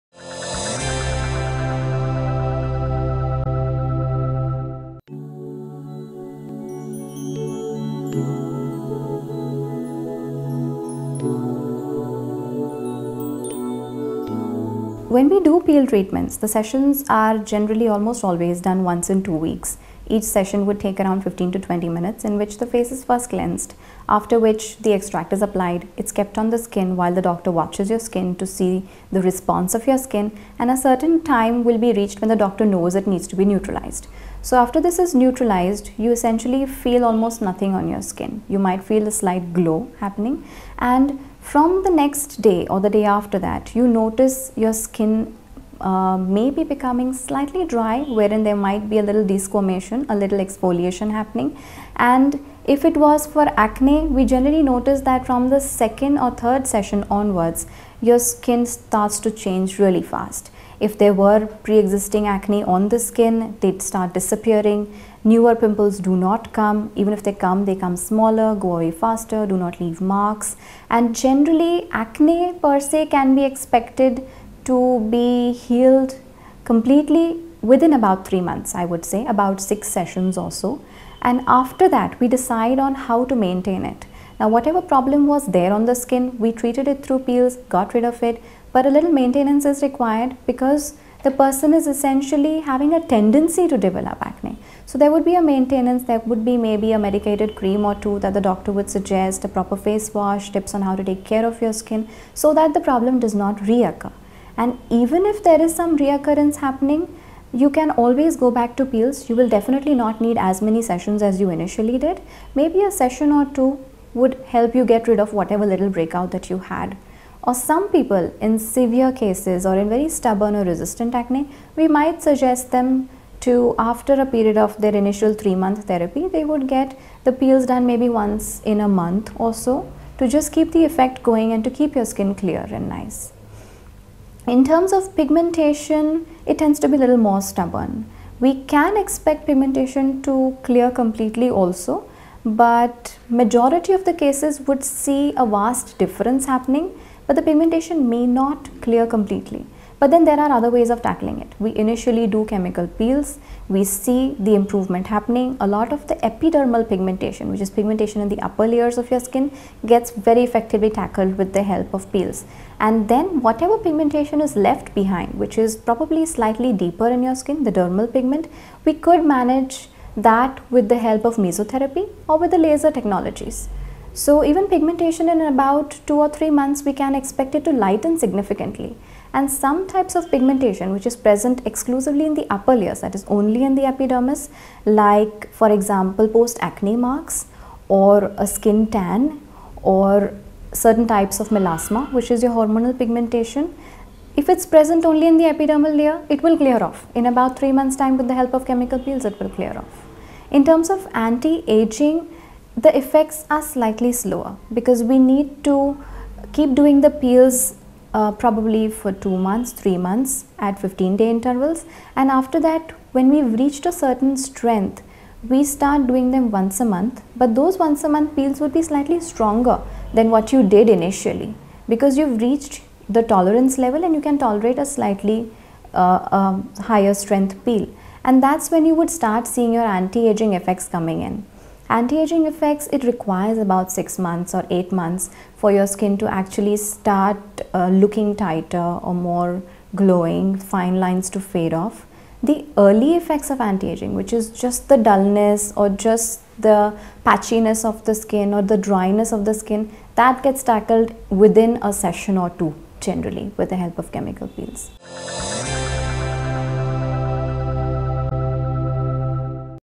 Duration of Chemical Peel Treatment - Onlymyhealth.com
Peel treatments are generally done once in two weeks. In a normal peel treatment session, firstly, the skin is cleansed, and the extract is applied. It is then neutralized after a certain time, and after a day or two of neutralization the skin may become dry and exfoliate a little. In case of acne, the skin starts responding from the second or third session and the existing acne start disappearing and newer pimples are less likely to appear. After a peel session some maintenance (a face wash or a medicated cream) might be needed as the person may redevelop acne.  Pigmentation on the other hand is more rigid to peel treatment where the epidermal pigmentation clears a lot however some deeper pigmentation is left under the skin, which may require mesotherapy or laser technology to lighten. Peels are effective, though slower in case of anti aging. -Dr. Niketa Sonavane, (Dermatologist, Dr. Niketa's Skin Secrets).